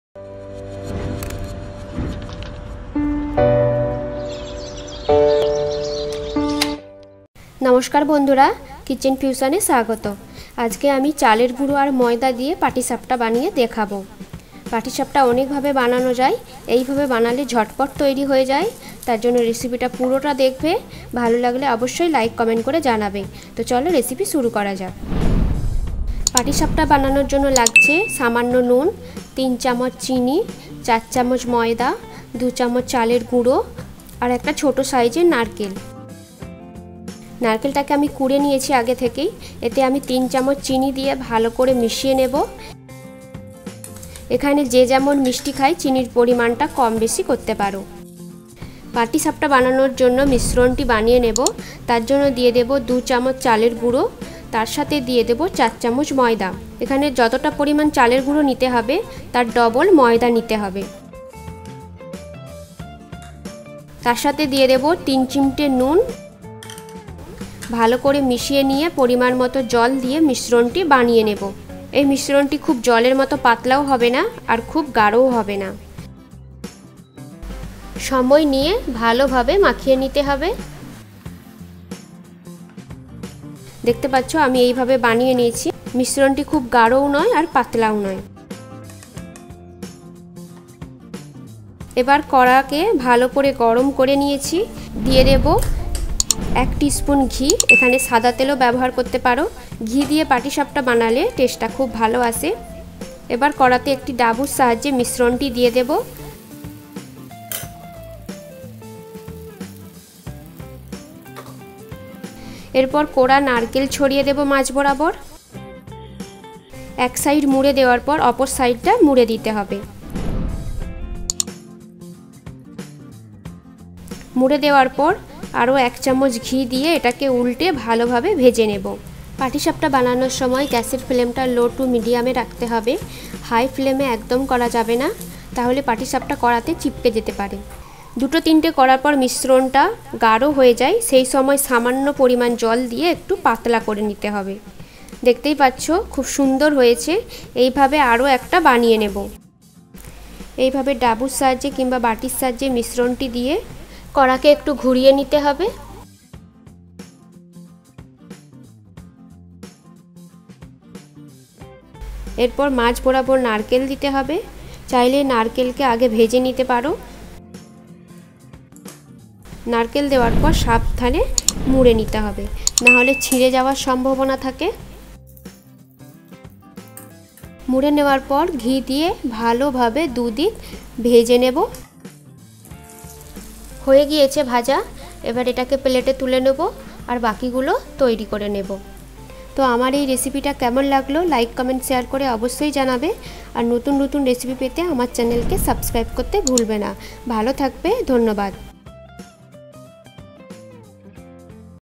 Patishapta Pitha Recipe In Bengali | Sweet Pitha Recipe - Homemade Sweet
Patishapta Pitha Recipe In Bengali. Sweet Pitha Homemade Recipe Easily.This is a traditional Bengali pancake recipe where the pancake are stuffed with Kheer or khowa

এটা আমাকে নতুন নতুন রেসিপির ভিডিও বানাতে উৎসাহ যোগাই।